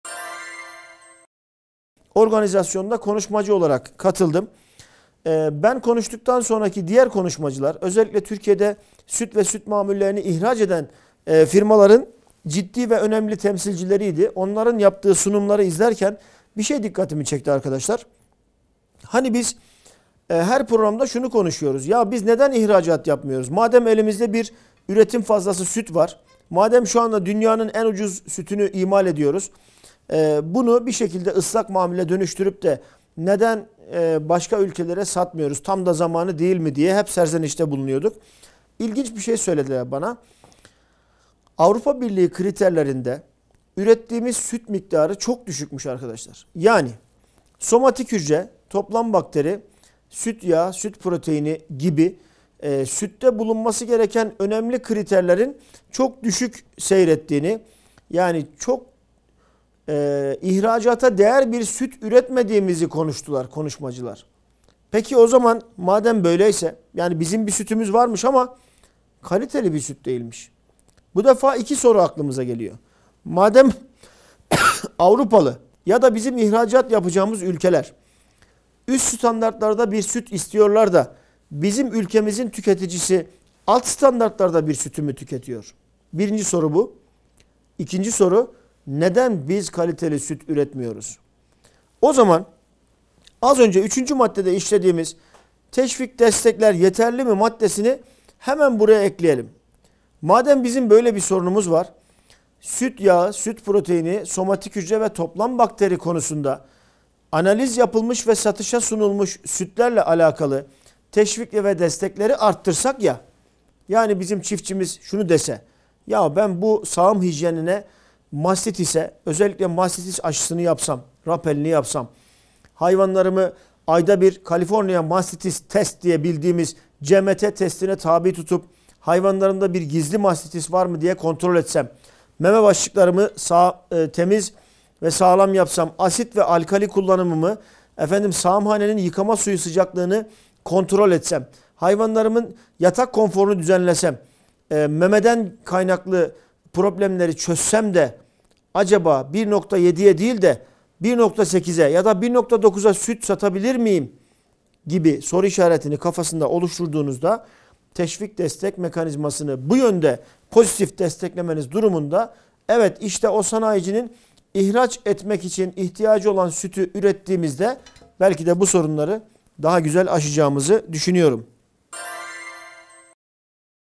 Hayvancılık Gündemi | Üretim Fazlası Sütü Neden İhraç Edemiyoruz?
Hayvancılık Gündemi programında bu hafta; Eşref Şekerli sizden gelen soruları cevaplandırıyor.